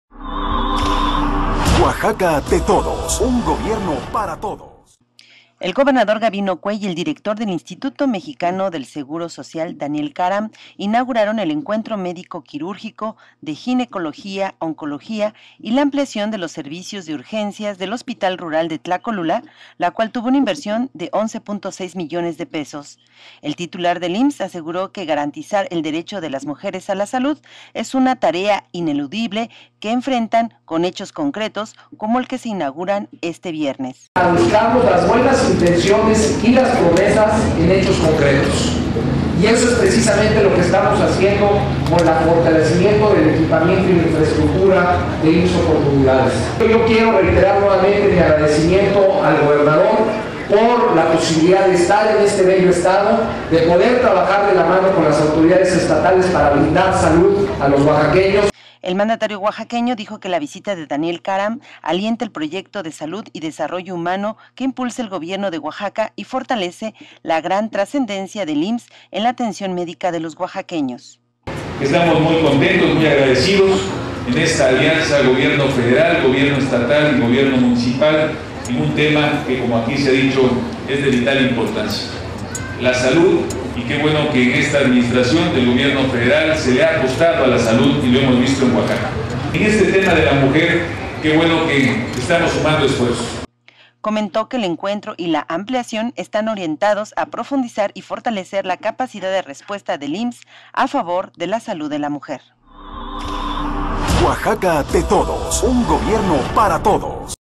Reconoce IMSS proyecto de salud del
Tlacolula de Matamoros, Oax.- El director general del Instituto Mexicano del Seguro Social (IMSS), Daniel Karam Toumeh reconoció aquí el proyecto de salud del Gobierno de Oaxaca que encabeza Gabino Cué, y que las familias oaxaqueñas pueden sentirse seguras en materia de salud, porque cuentan con todo el respaldo del Presidente Felipe Calderón.

En el marco del Día Internacional de la Mujer, el gobernador Gabino Cué Monteagudo y el funcionario federal inauguraron aquí el Encuentro Médico Quirúrgico de Ginecología Oncológica y la ampliación de los servicios de urgencias del Hospital Rural de Tlacolula, la cual tuvo una inversión de 11.6 millones de pesos.

En presencia de la edil, Concepción Robles Altamirano, el funcionario federal indicó que es obligación del Estado Mexicano acercar los programas de salud a las familias oaxaqueñas de manera cálida y humana "y puede seguir contando con todo nuestro apoyo y tengan por seguro que no les vamos a fallar".

Indicó que por instrucciones del Presidente Felipe Calderón, el IMSS tiene la encomienda de estar muy cerca de las mujeres oaxaqueñas, por lo que en la inauguración de estos servicios de urgencia y encuentro médico se utilizarán equipos con tecnología de punta y personal especializado proveniente de la Ciudad de México.

           Al respecto, el gobernador Gabino Cué reconoció la visita de Daniel Karam, pues alienta el proyecto de salud y desarrollo humano que impulsa el gobierno de Oaxaca, y desde luego, fortalece la gran trascendencia que el IMSS tiene para hacer efectivo el derecho a la salud de los oaxaqueños.

             Comentó que el encuentro y la ampliación están orientados a profundizar y fortalecer la capacidad de respuesta del IMSS a favor de la salud de la mujer, la cual se suma a los programas que impulsan su gobierno a través del Programa Bienestar y el servicio que presta el Gobierno del Estado a través de la ambulancia aérea.

Tlacolula de Matamoros, Oax., 9 de marzo de 2012